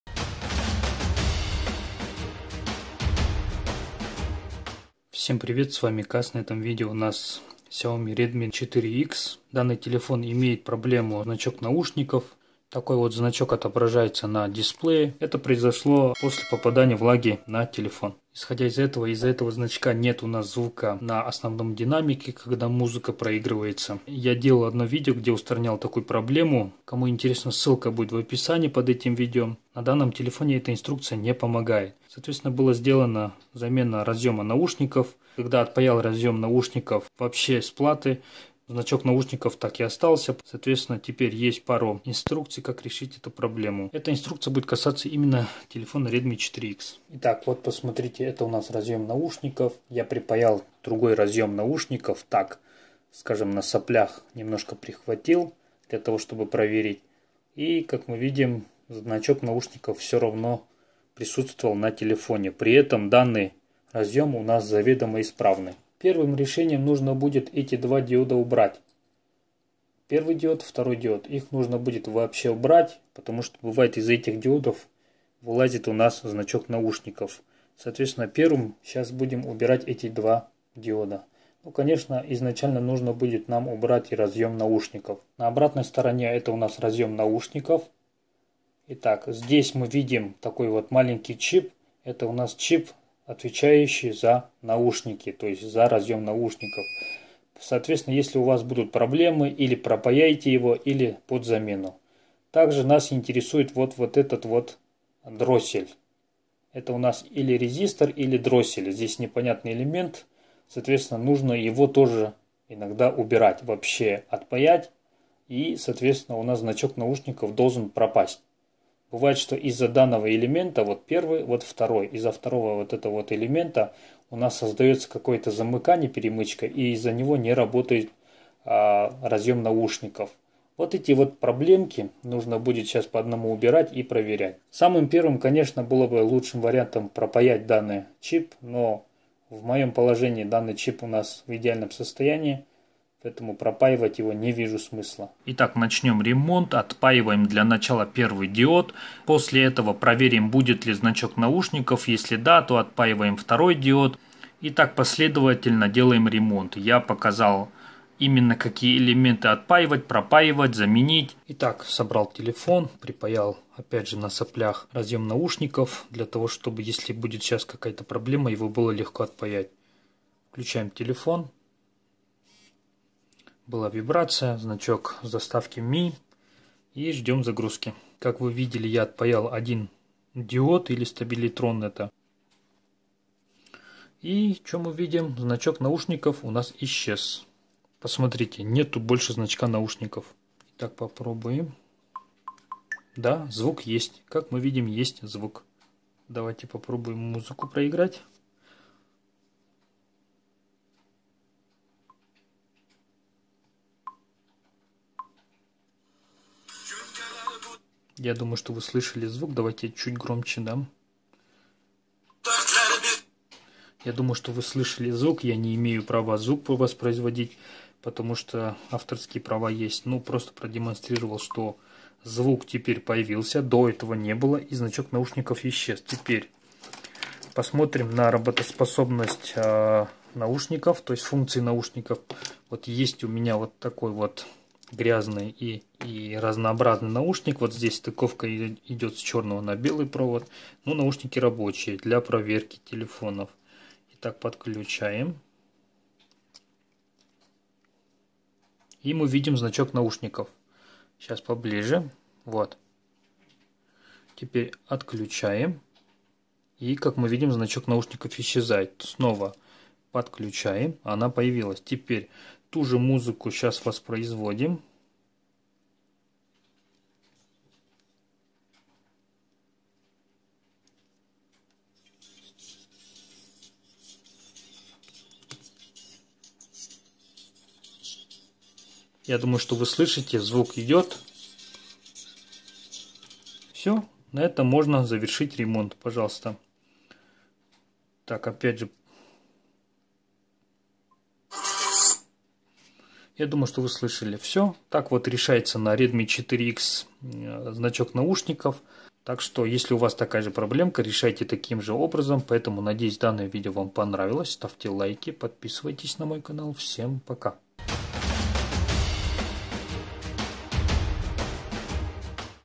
Значок наушников на телефоне - Xiaomi redmi 4x, нет звука
На этом видео телефон Xiaomi redmi 4x, появился значок наушников, нет звука. При этом наушники не подключены к телефону.

► Мы в Инстаграмме - @cas_channel

► Если ссылки не рабочие дайте пожалуйста знать
► Желающим поддержать проект: